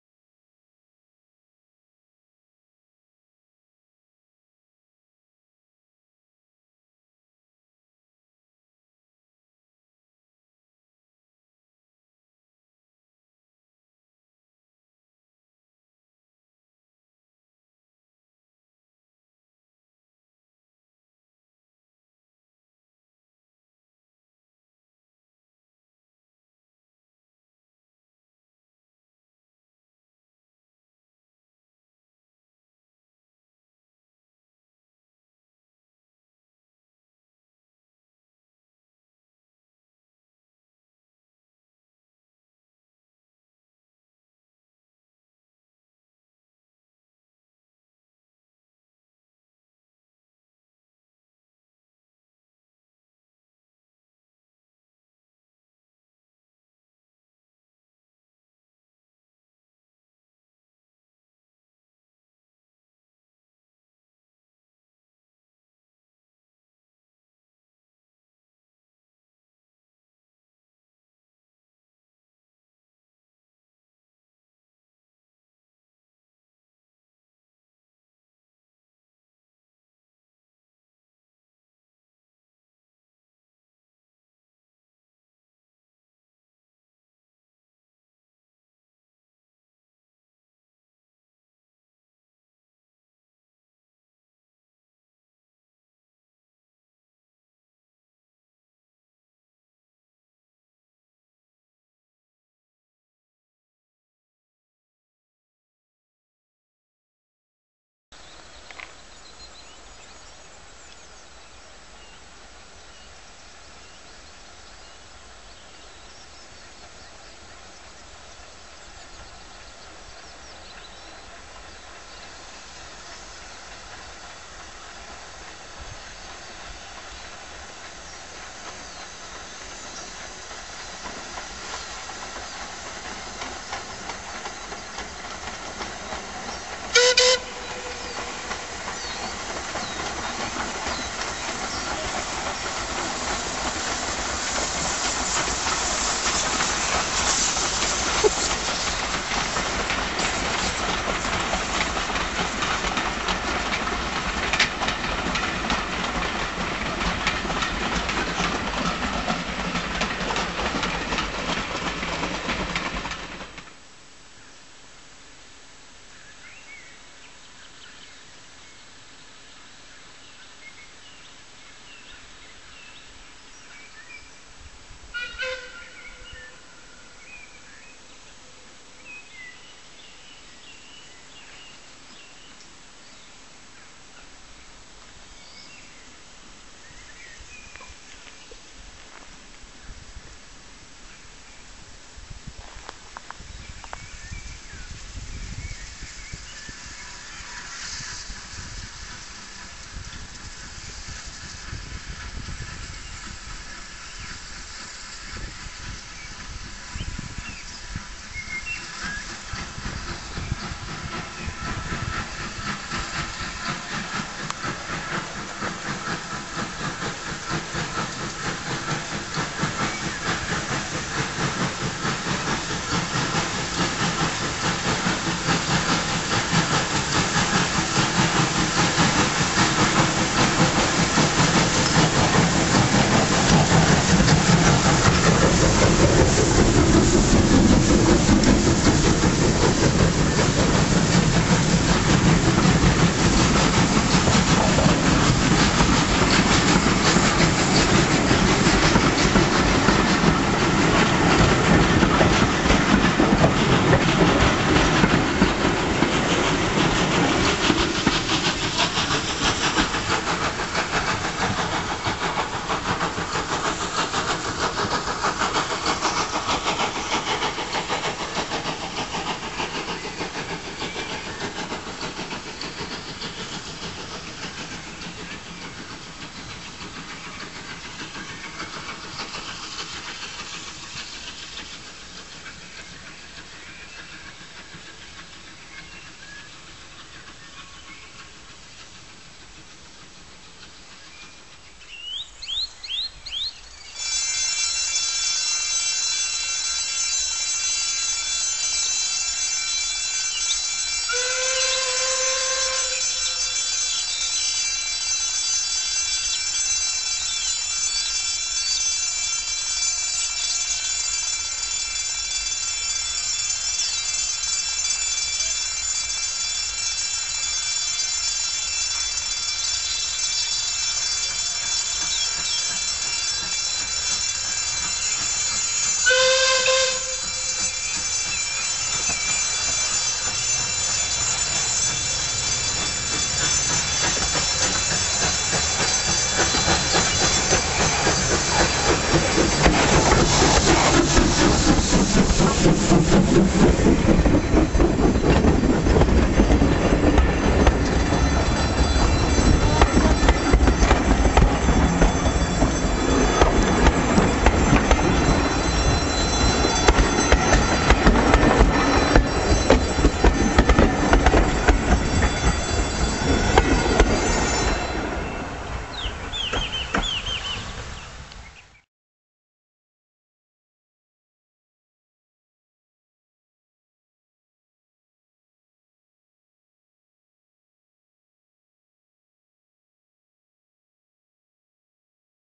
Caley 828's Return on the Strathspey Railway.
On 14th June, Caledonian Railway 812 class number 828 was unveiled for the public for the first time since being withdrawn from traffic in 2002 after a heavy overhaul costing £220 000. Resplendent in magnificent Caledonian Blue livery, she made her first passenger run on the afternoon of 16th June, then hauled that evenings Strathspey Highlander Wine and Dine special.